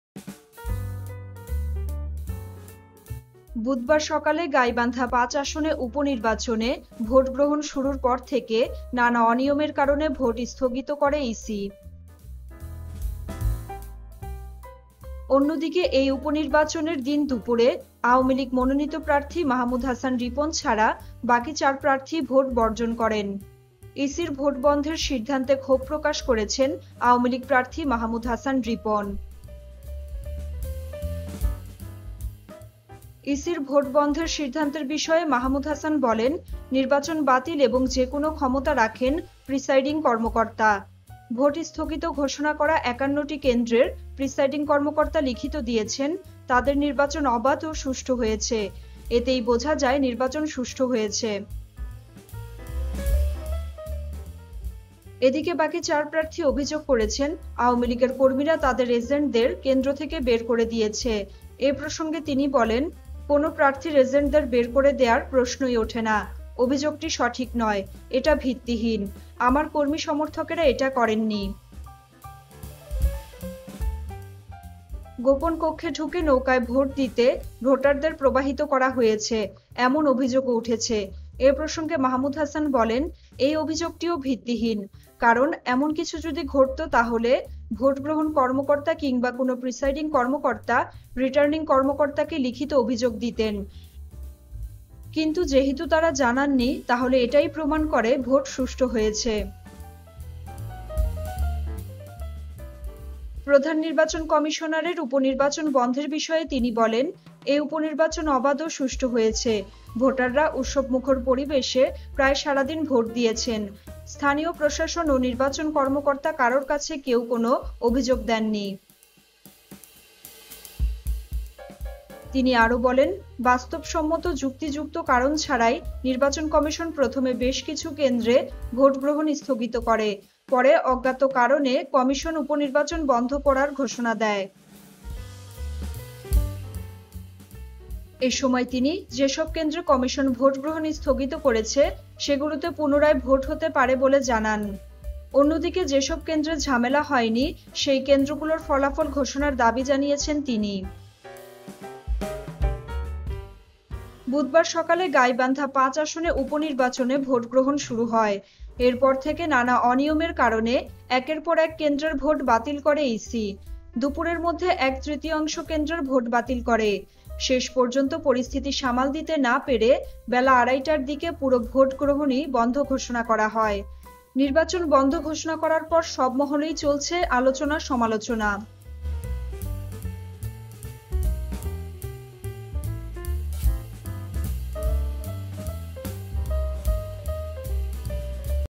গাইবান্ধায় ভোট নিয়ে যা বললেন আওয়ামী লীগ প্রার্থী মাহমুদ হাসান রিপন! | Ajker Saradin
গাইবান্ধায় ভোট নিয়ে যা বললেন আওয়ামী লীগ প্রার্থী মাহমুদ হাসান রিপন! নিউজটি ভালো লাগলে অবশ্যই একটা লাইক দিন, কমেন্টে আপনার মতামত জানান ও শেয়ার করে সবাইকে দেখার সুযোগ করে দিন ।।